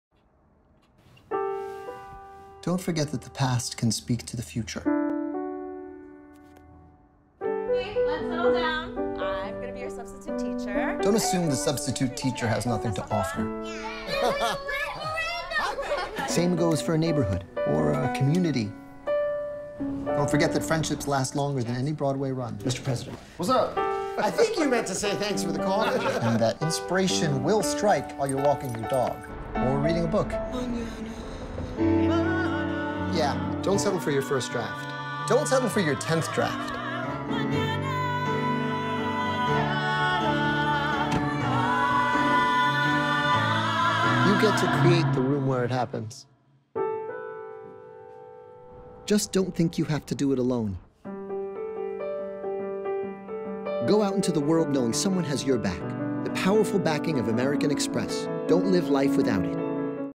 Lin-Manuel Miranda teams up with American Express 60s
Lin-Manuel Miranda, creator of Hamilton and star of the upcoming movie, Mary Poppins Returns, has teamed up with American Express in a campaign designed to assist small businesses in the communities that were most impacted by the recent storms across Texas and Florida.